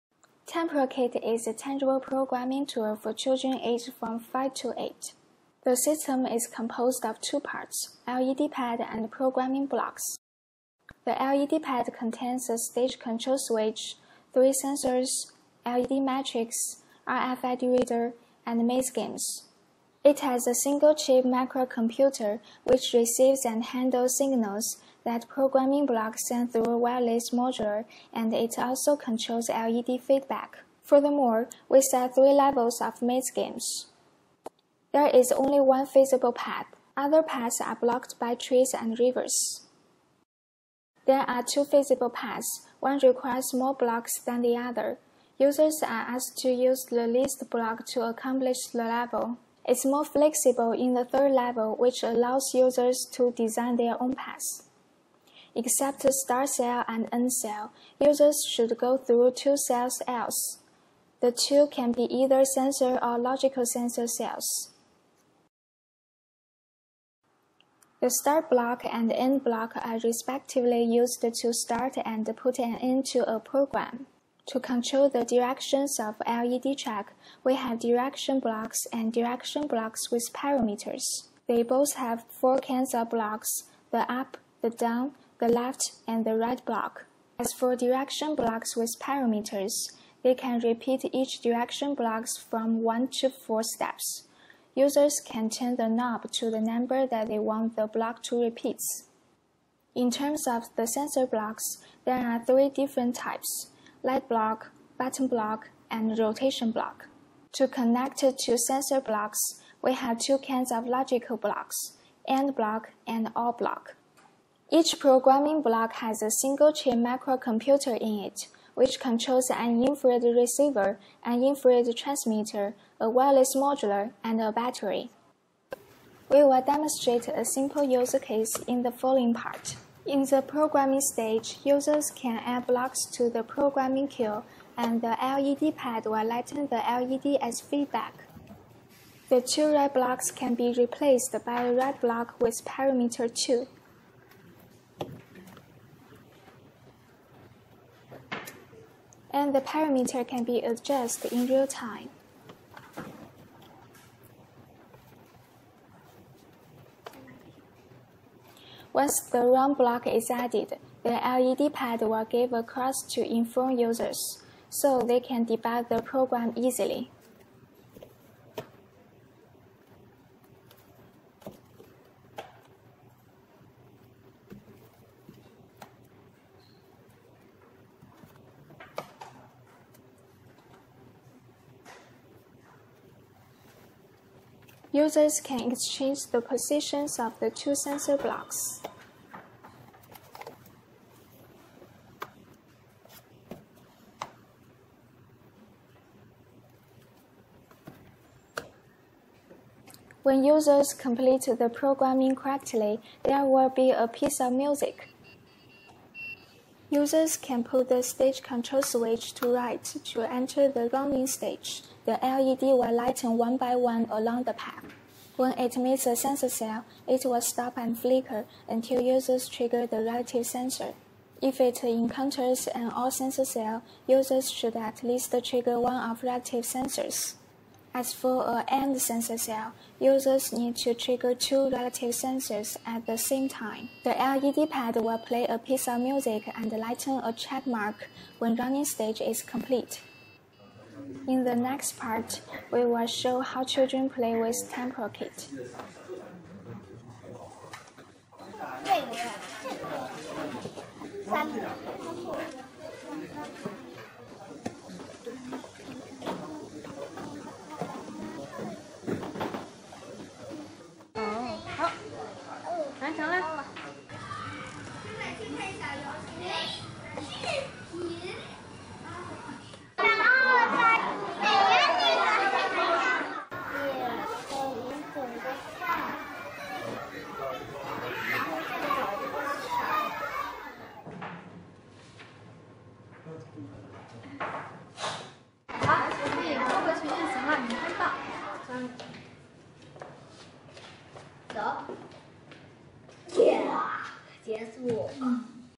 TanPro-Kit: A Tangible System to Help Children Learn Programming Concepts
TanPro-Kit is a low-cost, tangible system designed for children aged 5 to 8 to learn programming concepts. The system includes two major components: programming blocks, with which children play maze games based on system instructions, and a LED Pad, which uses animations and sounds to give children feedback on how well they arrange blocks in a game.